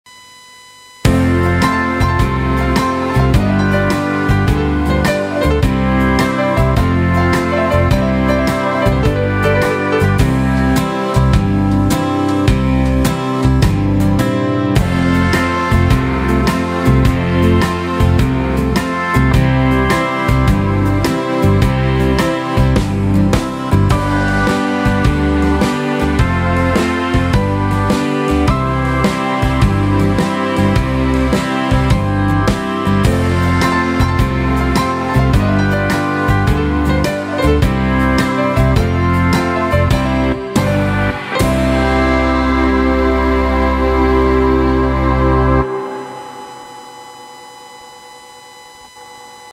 23 Country Swing BPM 105
1, Some of the DJ Styles has been removed. (In the Previous Yamaha Keyboards)
2. Some of the Style name has changed to something else!
3. Some of the Style has a Different Variation then the old ones!
4. Some of the NEW Styles has been added!
One more thing,  On the Entertainer Style No.50 Akkoredon Polka hasn't Revealed yet!
It'll have to revealed the demo style really soon!